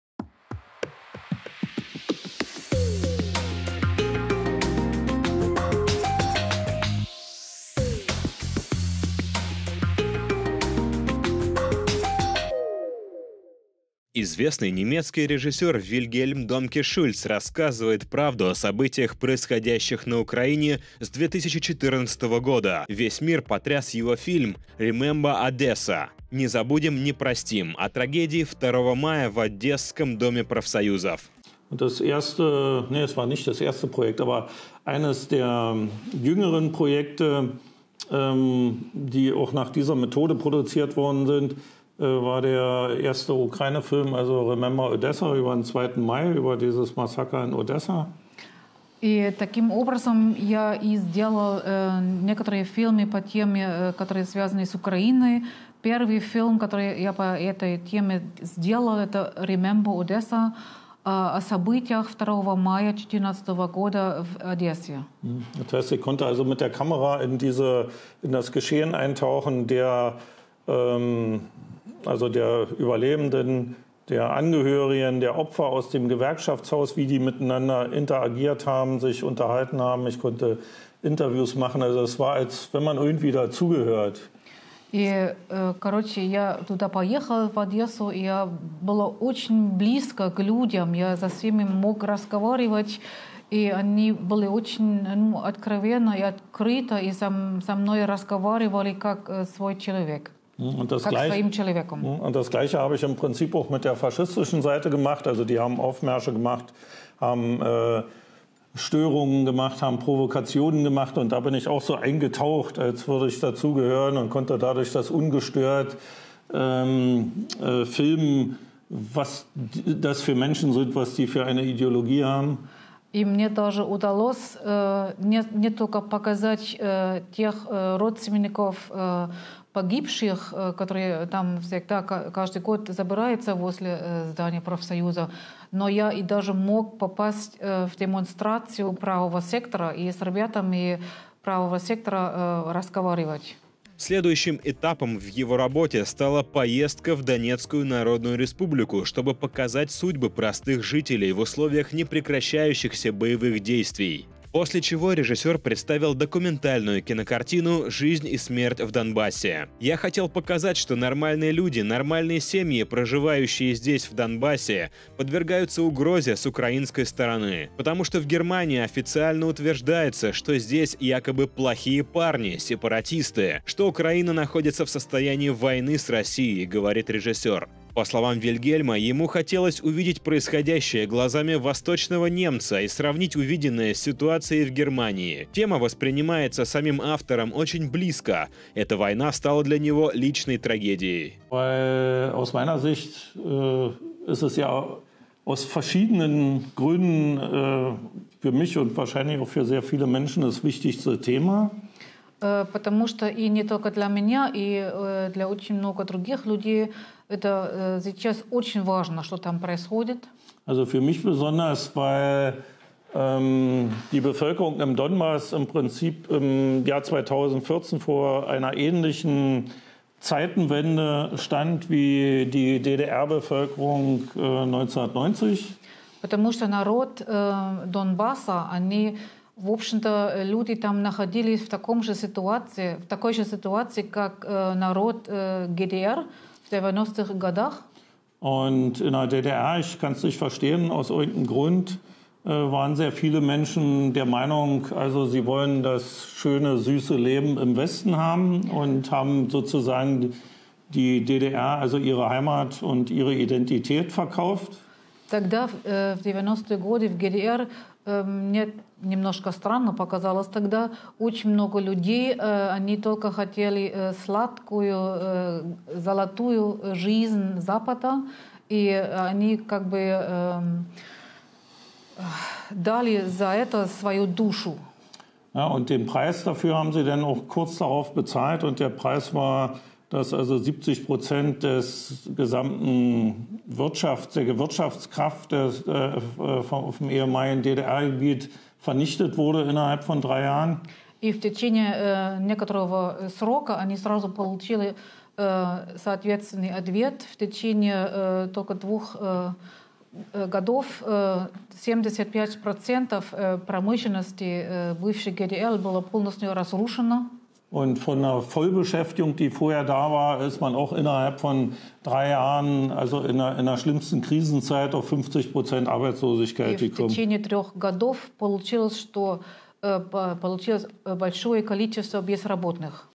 Немецкий режиссер рассказывает правду о событиях на Украине
Известный немецкий режиссер Вильгельм Домке-Шульц рассказывает правду о событиях, происходящих на Украине с 2014 года. Весь мир потряс его фильм «Remember Odessa - не забудем, не простим» о трагедии 2 мая в одесском Доме профсоюзов.